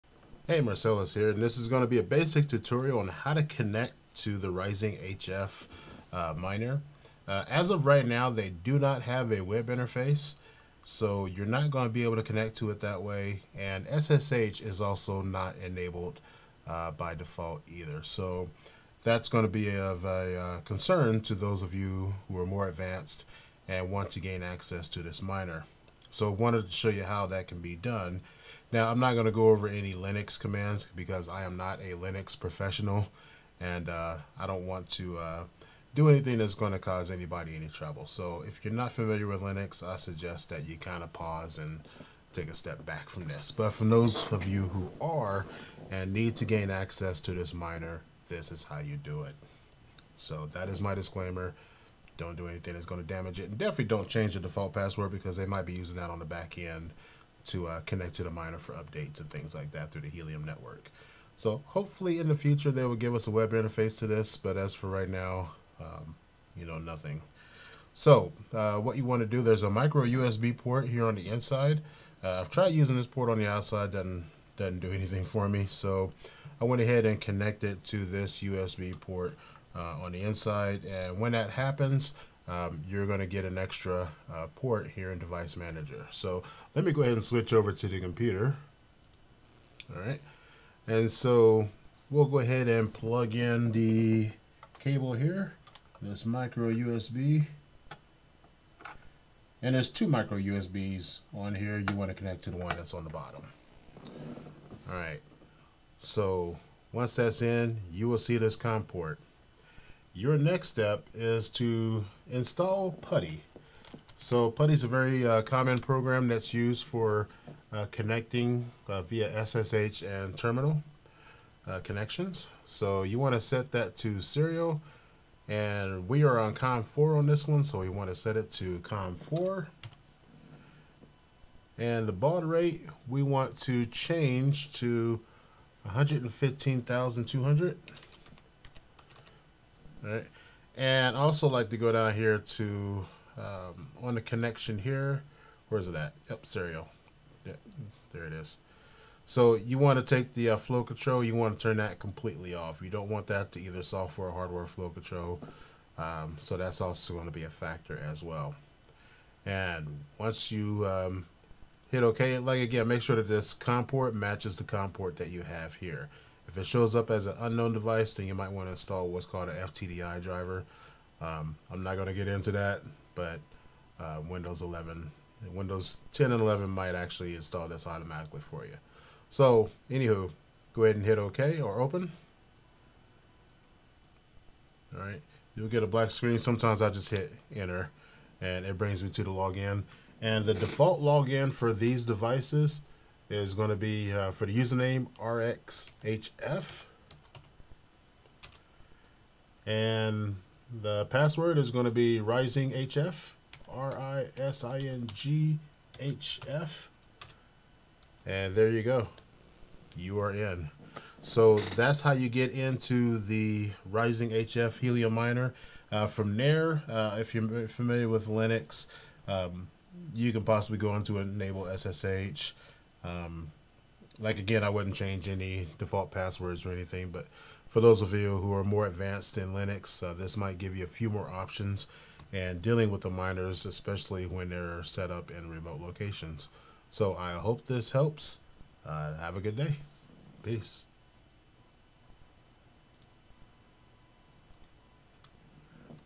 How to connect to the RisingHF Helium miner
Being a bit frustrated with the RisingHF's lack of a user interface or way to connect to the hardware, despite what the product documentation says, I wanted to provide a basic how-to video on how to connect to miner via a serial connection using the USB Port